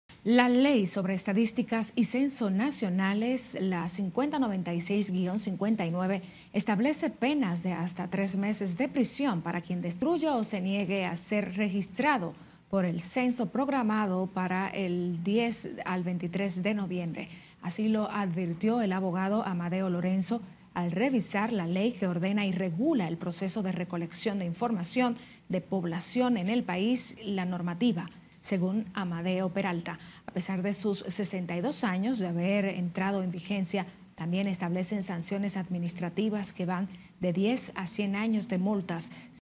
Podrían enfrentar tres meses de prisión personas se nieguen a ser censados
La Ley sobre Estadísticas y Censo Nacionales la 5096-59 establece penas de hasta 3 meses de prisión para quien obstruya o se niegue a ser registrado por el censo programado para del 10 al 23 de noviembre.

Así lo advirtió el abogado Amadeo Lorenzo al revisar la ley que ordena y regula el proceso de recolección de informaciones de población en el país.